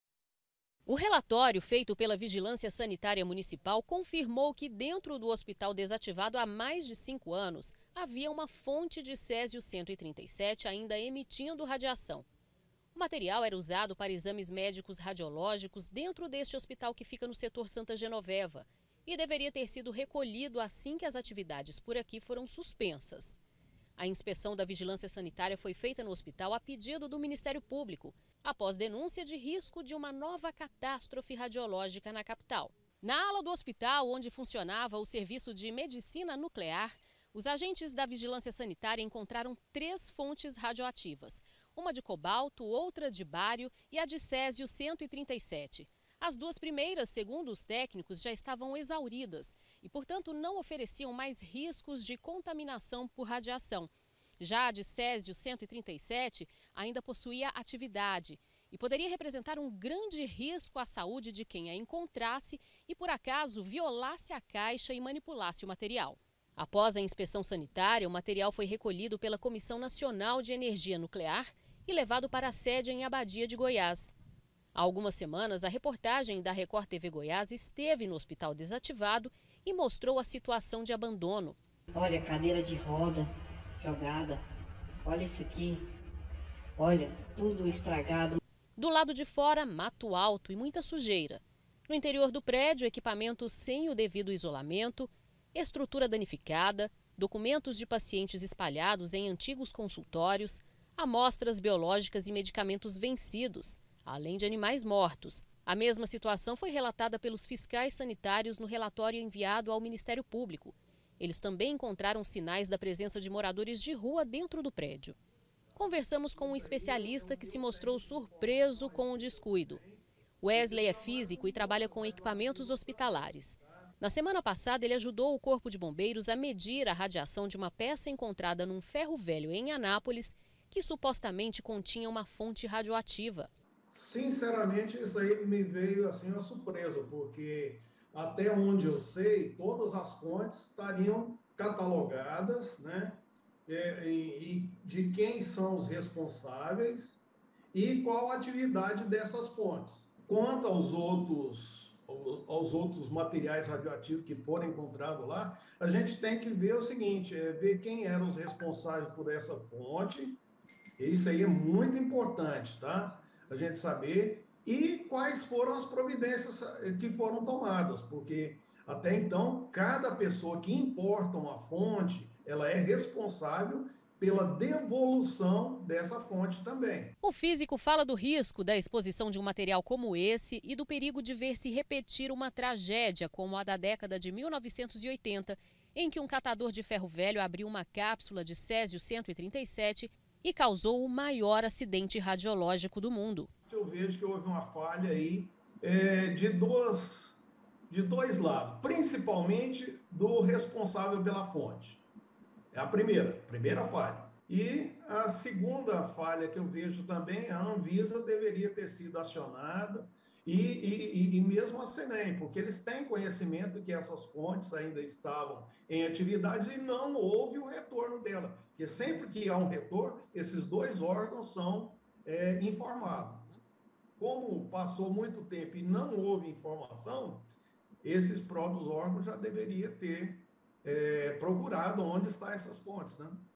RISCO À SAÚDE: INSPEÇÃO ENCONTRA CÉSIO EM HOSPITAL ABANDONADO
O RELATÓRIO DA VIGILÂNCIA SANITÁRIA, OBTIDO COM EXCLUSIVIDADE PELO JORNALISMO DA RECORD TV GOIÁS, TRAZ UMA INFORMAÇÃO ASSUSTADORA. FOI ENCONTRADA UMA CAPSULA DE CÉSIO 137, NO HOSPITAL QUE ESTÁ ABANDONADO HÁ MAIS DE CINCO ANOS, NO SETOR SANTA GENOVEVA, EM GOIÂNIA. A FONTE AINDA TINHA ATIVIDADE, E CASO FOSSE VIOLADA POR ALGUÉM, PODERIA PROVOCAR SÉRIOS RISCOS A SAÚDE DA PESSOA.